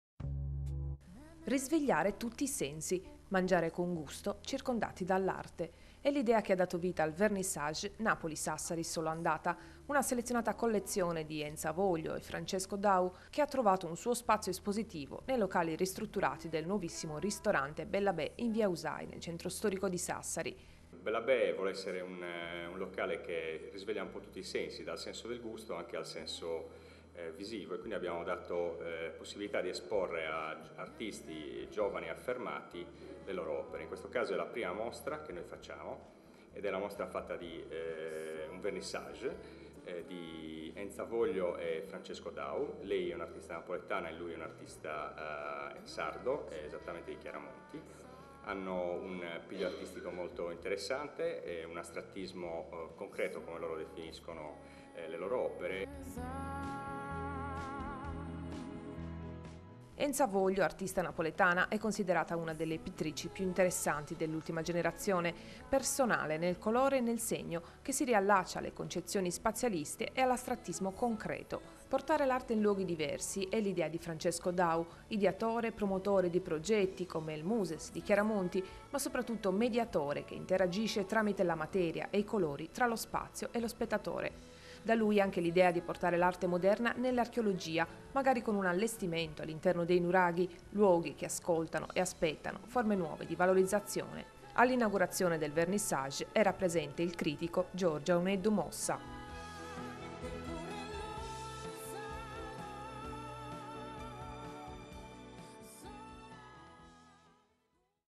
Vernissage al Bella Bè di Sassari
Vernissage di Enza Voglio e Francesco Dau al ristorantino Bella Bè di via Usai, a Sassari. Il titolare, Lele Ciaravola, ci parla della sua idea: unire il gusto dell'arte al gusto del cibo. Servizio di Giulia Bardanzellu del Tg Cinquestelle del primo aprile 2010.